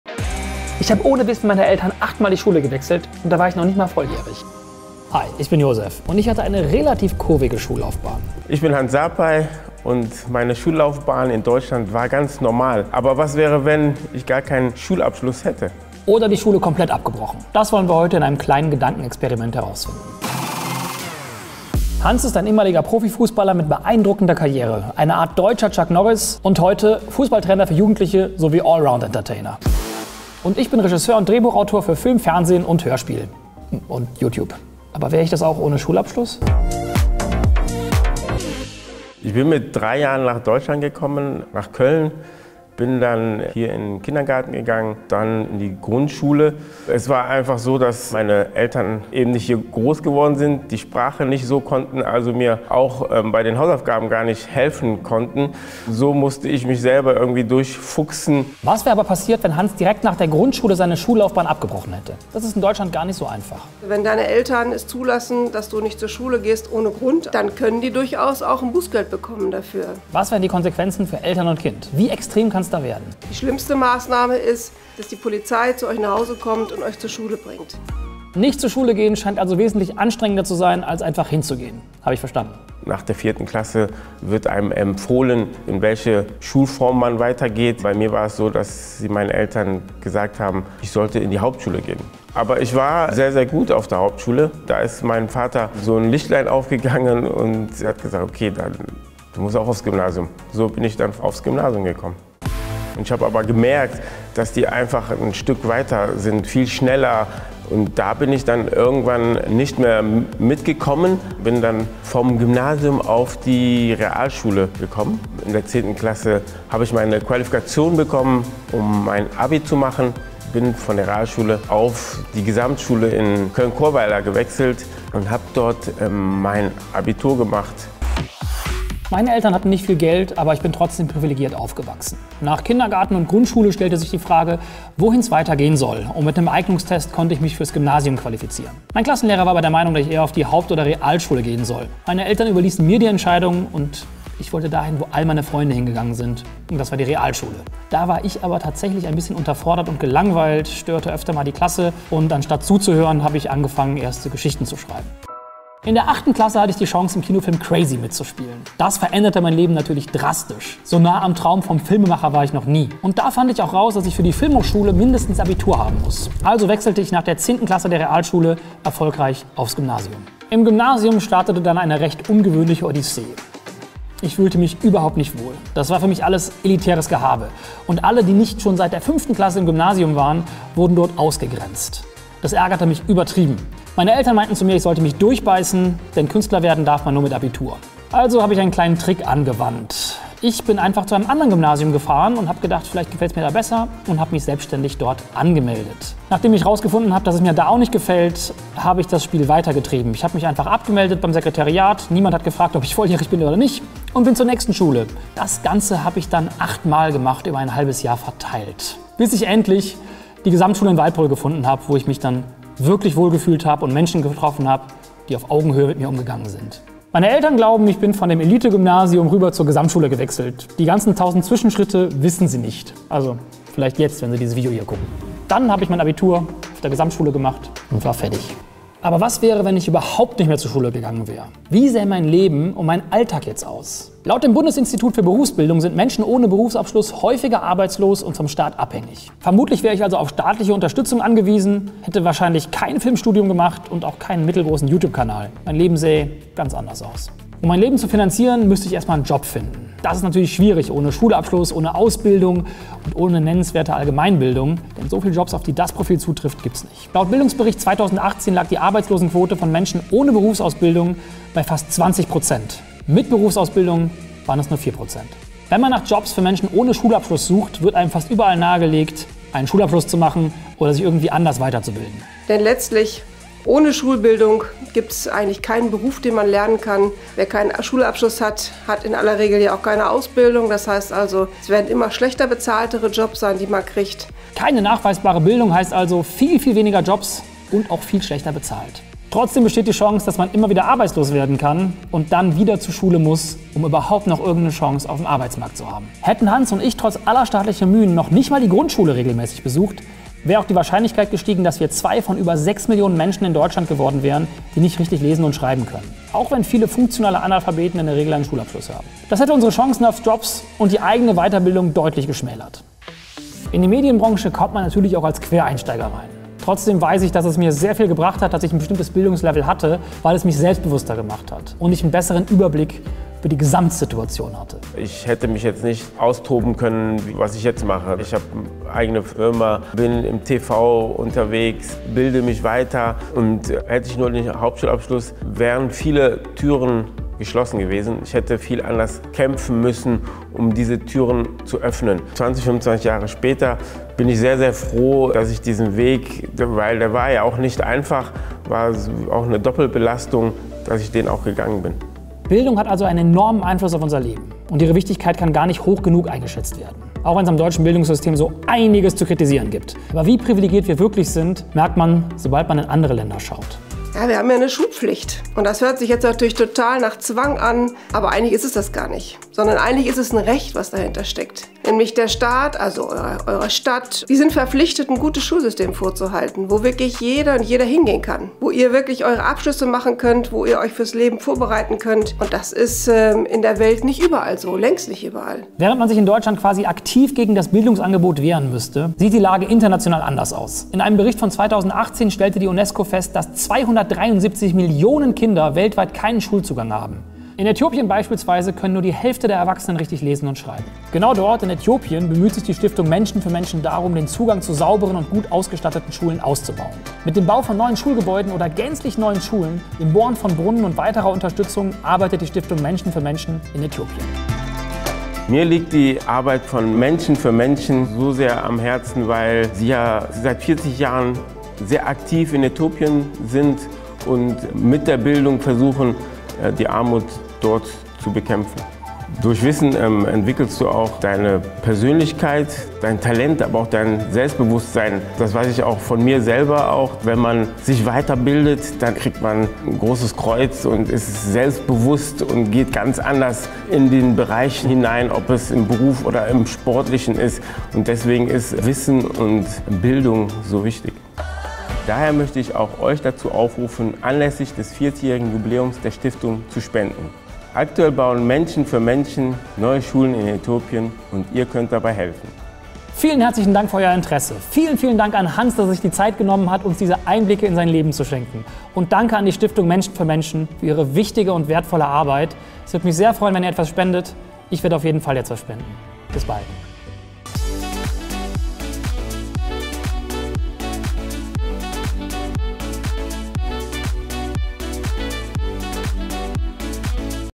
Leben ohne Schule? | Gedankenexperiment mit Hans Sarpei | 4K
🧠 Schaut unbedingt bei MenschenFürMenschen vorbei und wenn ihr könnt, unterstützt diese wichtige Arbeit (Werbung)

💛 ein paar worte von joseph➡️
Ich hatte schon länger vor ein Gedankenexperiment über meine kurvige Schullaufbahn zu machen und wollte wissen, wie extrem es wirklich werden kann, wenn man einfach die Schule abbricht. Sind die Auswirkungen wirklich so krass?
Als die Anfrage von Menschen für Menschen kam, war klar, dass die Idee des Videos und die Mission der Stiftung gut zusammen passen, um dieses Gedankenexperiment endlich anzugehen.

🤐 Zum Video:
Gemeinsam mit Hans Sarpei gehe ich der Frage nach, was passiert wäre, wenn wir beide an verschiedenen Stellen unserer Schullaufbahn die Schule abgebrochen hätten. Was wären die Konsequenzen gewesen? Wo würden wir heute stehen? Wäre Hans trotzdem  Profi-Fußballer und ich Regisseur geworden?
Als Expertin steht Maike Finnern von der Gewerkschaft Erziehung und Wissenschaft (GEW) zur Seite.

Und herzlichen Dank an die Erzbischöfliche Liebfrauenschule Köln für die Nutzung der Klassenräume

Skript: Dominik Hammes
Produktion: barracuda film GmbH

0:00 Hi
0:20 Grundschule
1:22 Weiterführende Schule
2:50 Acht Schulwechsel
3:49 Schulabbruch
4:56 Gar keine Schulbildung?
6:26 International
7:43 Danke an Menschen für Menschen

Die Musik in diesem Video ist ausnahmsweise mal von Artlist.